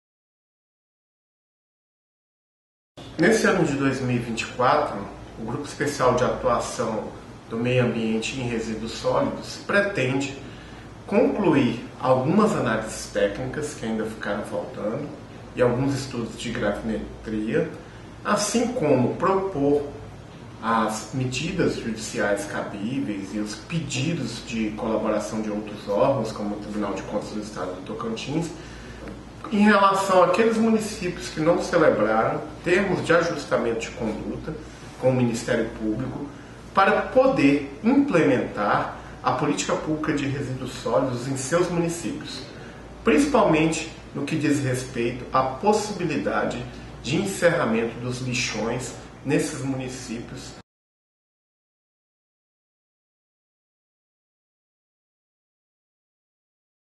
JTO VIDAURBANA LIXAO PROMOTOR
Promotor de justiça Francisco José Pinheiro Brandes Júnior, que atua na área ambiental do Ministério Público do Tocantins, explana ações do órgão ministerial para 2024